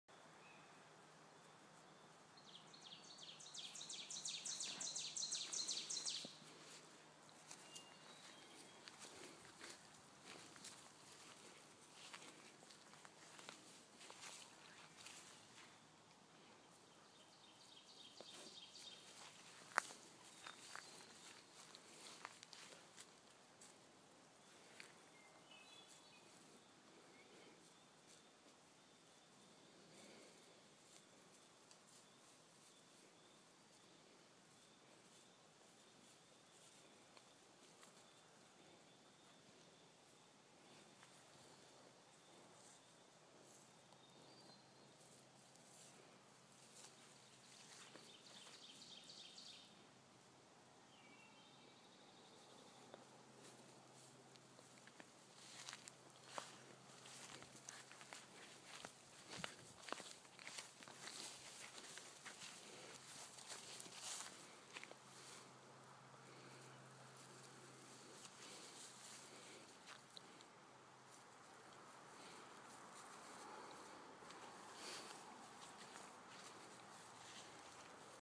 Freya and Pants
Freya feels better about her walks if another animal comes with us.  Her buddy Montey was adopted, so Pants steps in to fill the role of BFF and walking buddy.